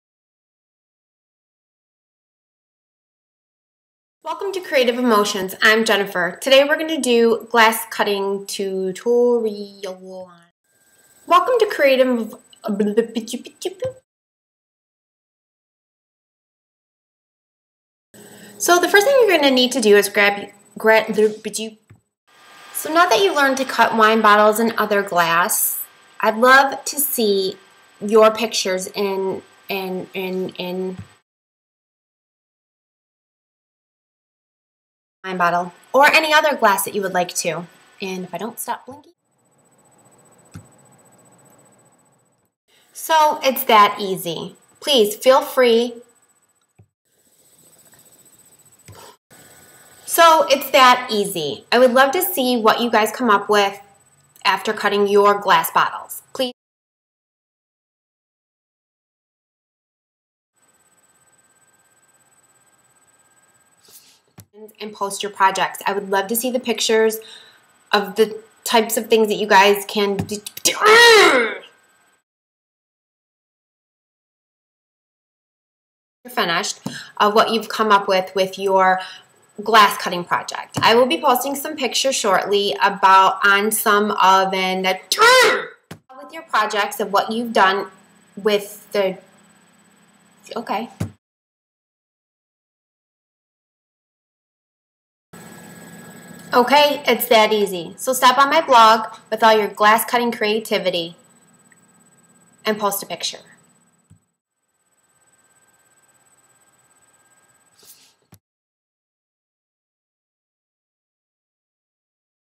Yep...That Just Happened!
This is my blooper reel from making my "How To" video. Go ahead and have a laugh... it's on me!!